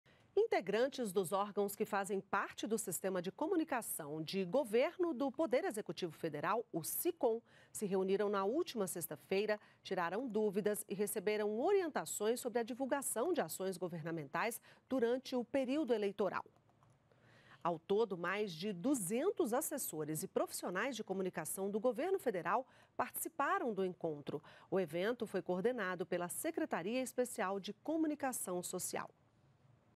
Comunicação | Orientações sobre eleições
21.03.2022 - Integrantes dos órgãos que fazem parte do Sistema de Comunicação de Governo do Poder Executivo Federal, o SICOM, se reuniram, na última sexta-feira (18), tiraram dúvidas e receberam orientações sobre a divulgação de ações governamentais durante o período eleitoral.